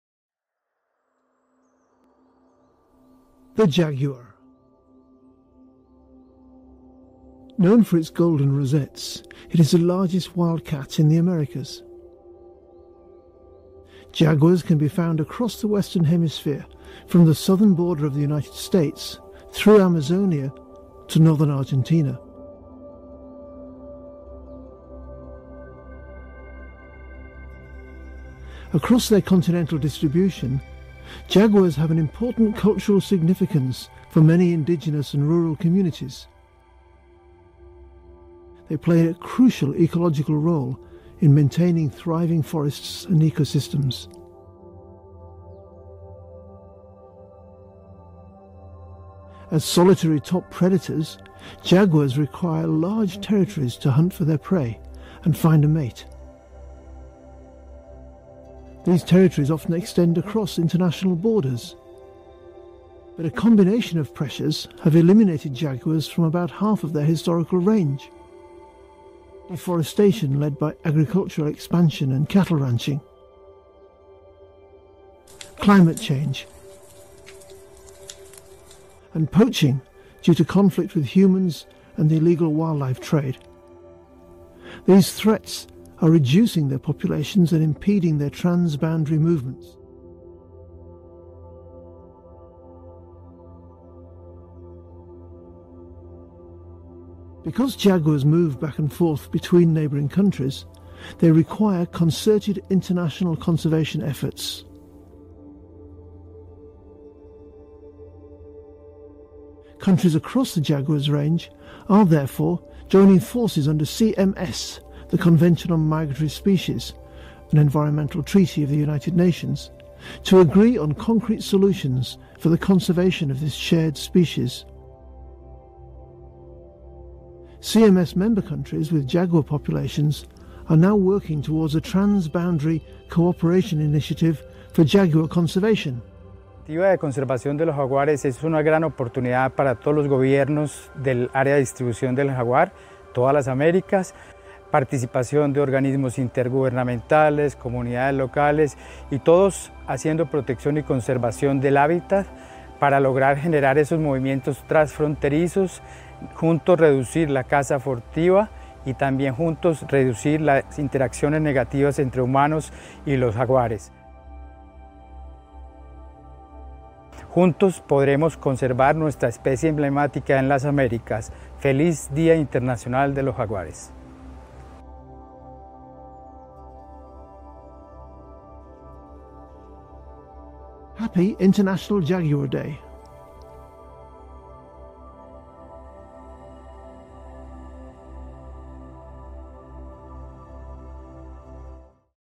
CMS Jaguar Initiative: A Lifeline for Big Cats & Biodiversity. Cross-Border Conservation in Action!
The Jaguar is a key predator and symbol of healthy ecosystems. Despite their vital role, Jaguars face severe threats like habitat loss, illegal trade, and human-wildlife conflict. Most subpopulations are Endangered or Vulnerable, highlighting the urgent need for cross-border conservation. 🐆

At CMSCOP14, the CMS Jaguar Initiative was launched to promote collaboration among Jaguar Range States. It aligns with the 2030 Jaguar Roadmap to protect habitats, reduce conflicts, and secure migration corridors. 🐾

Let’s work together to ensure a thriving future for Jaguars and their ecosystems! Learn more about the CMS Jaguar Initiative